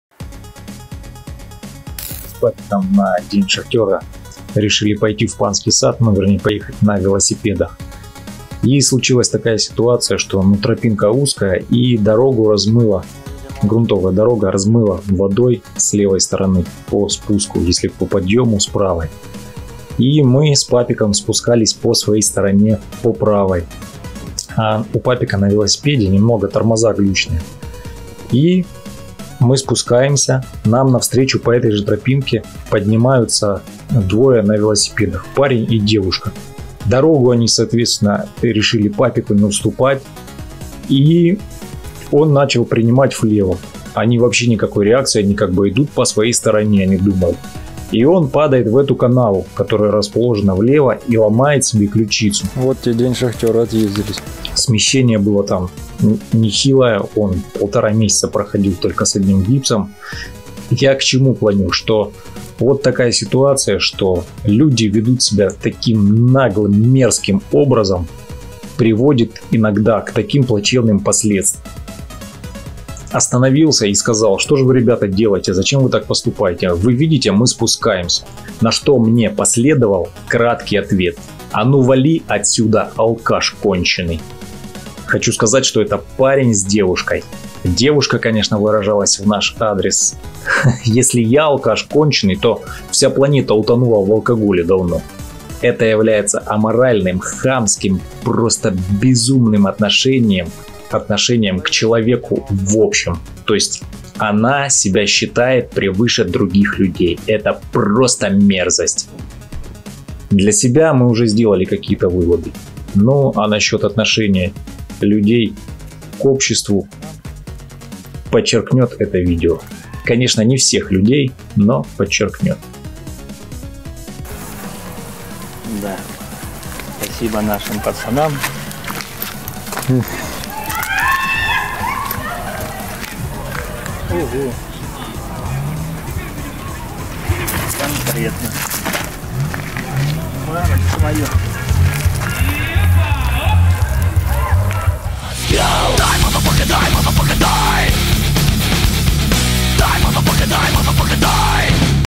СЛОМАЛ КЛЮЧИЦУ в день ШАХТЁРА
Приближается еще один день ШАХТЁРА и я надеюсь,что он будет не такой как в этом видео (в прошлый раз)